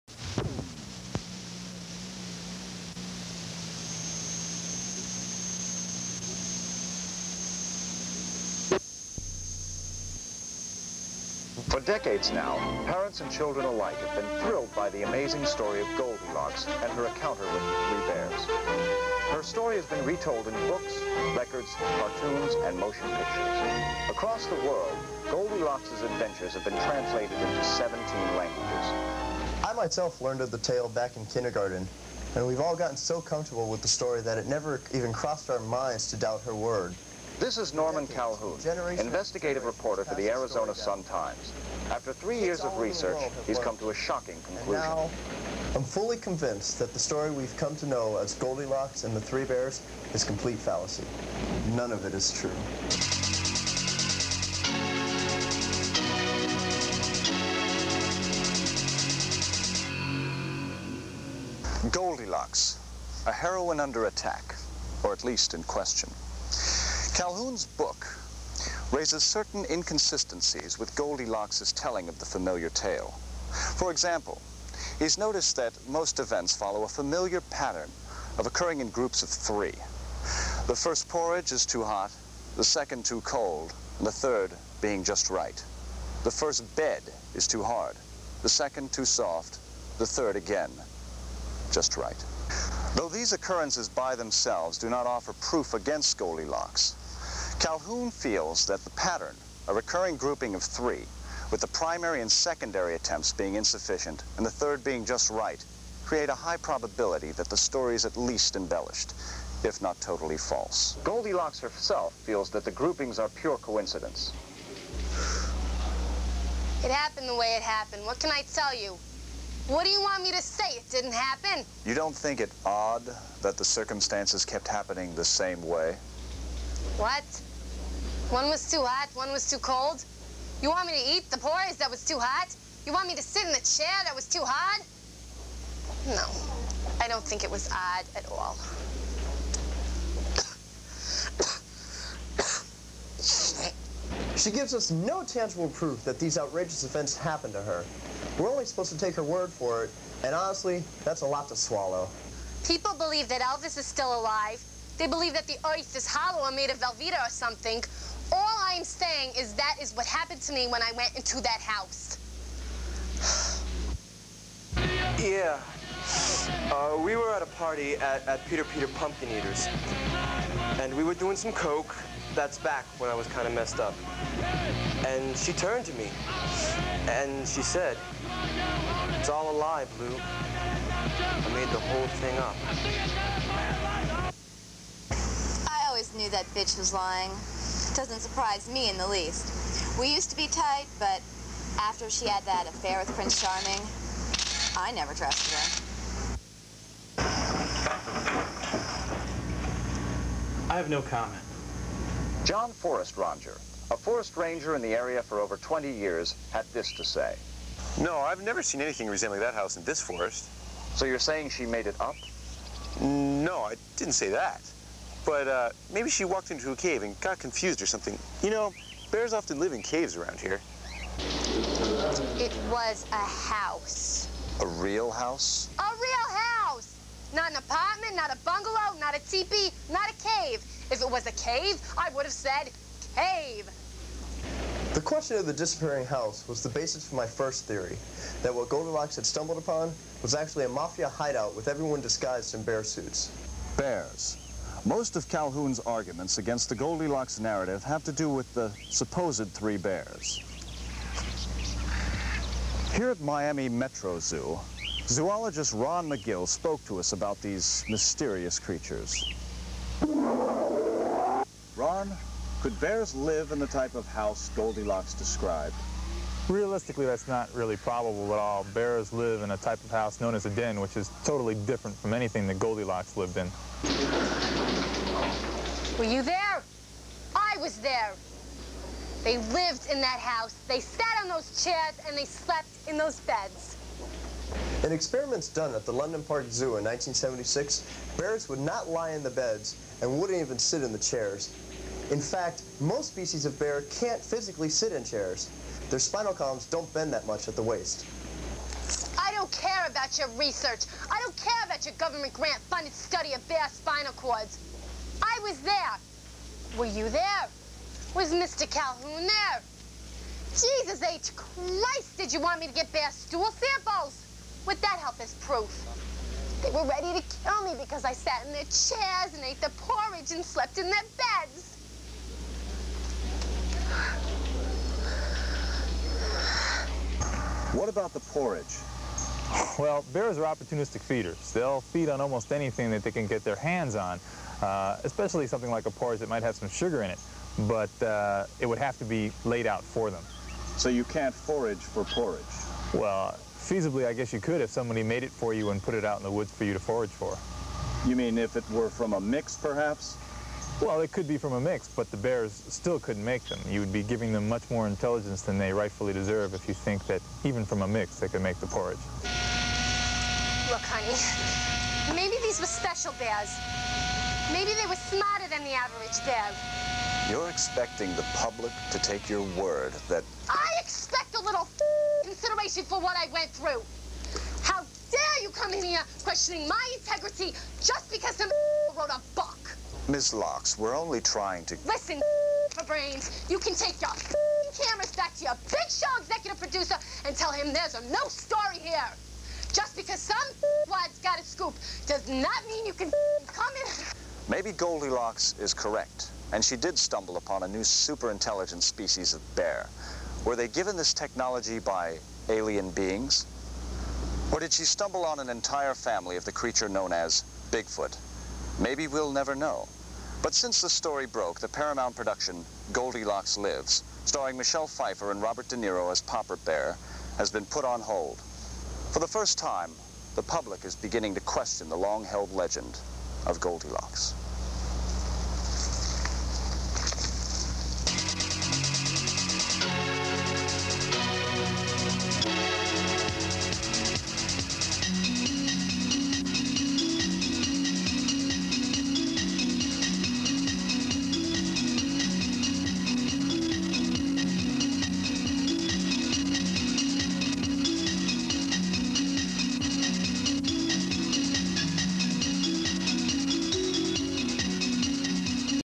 The Goldilocks Conspiracy
Did Goldilocks really encounter three disgruntled bears in a house in the woods?  Was the porridge really "too hot", "too cold" and "just right" as she alleges? Can bears really sleep in beds?  Or was it all just a fabricated and sensationalized ploy for fame, fortune and public sympathy? Does anyone care?  This hard-hitting documentary attempts to get the real story behind the age old fairy tale.  Directed by Steven Rosenthal and narrated by Rollin Jarrett.